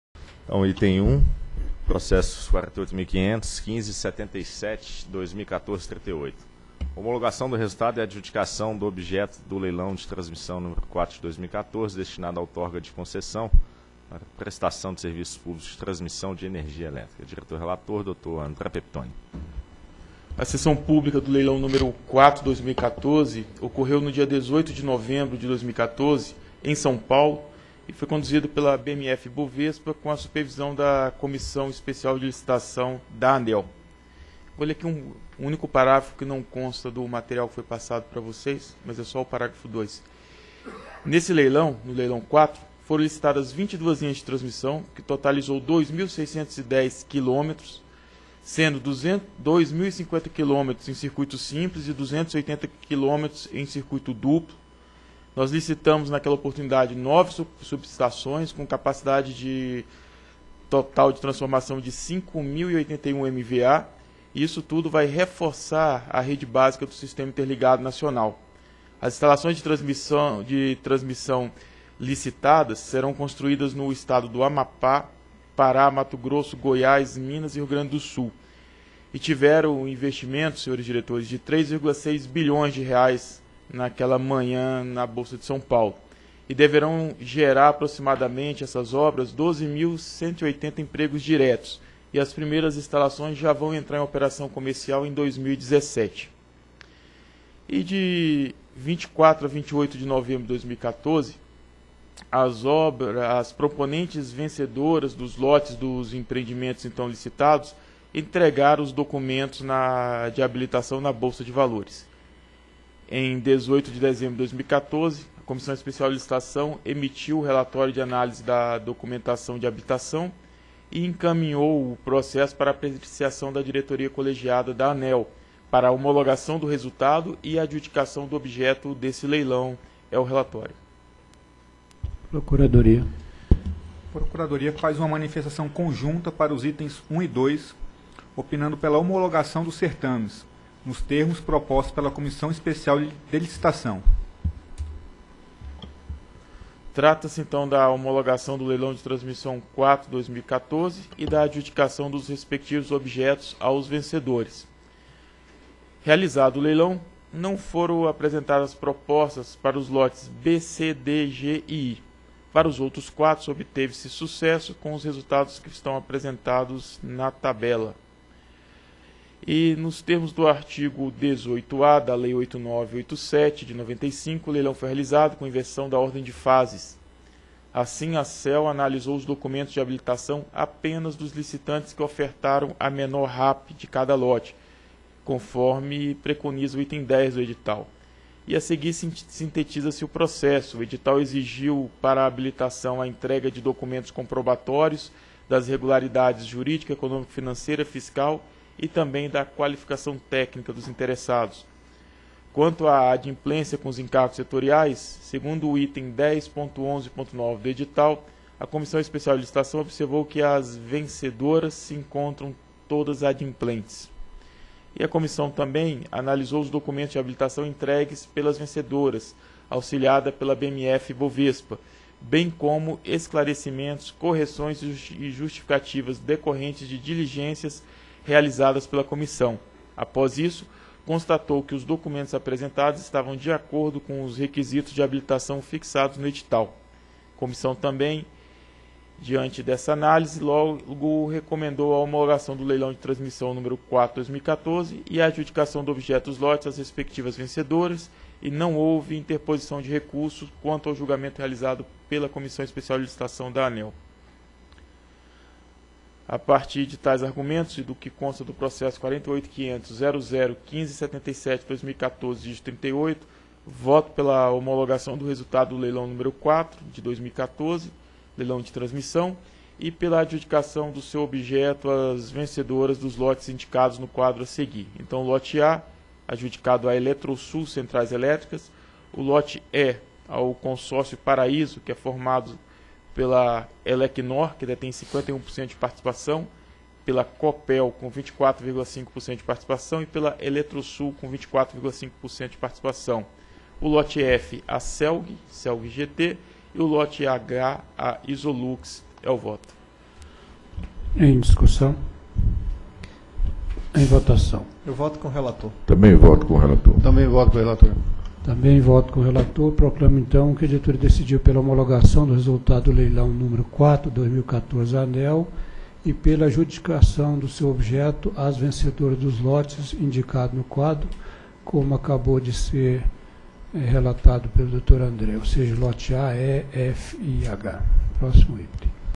Item 1 - 2ª Reunião Pública Ordinária ANEEL 27/01/2015
Item 1. Processo: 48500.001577/2014-38. Assunto: Homologação do resultado e adjudicação do objeto do Leilão de Transmissão nº 4/2014, destinado à outorga de concessão para prestação de serviços públicos de transmissão de energia elétrica. Relator: Diretor André Pepitone da Nóbrega.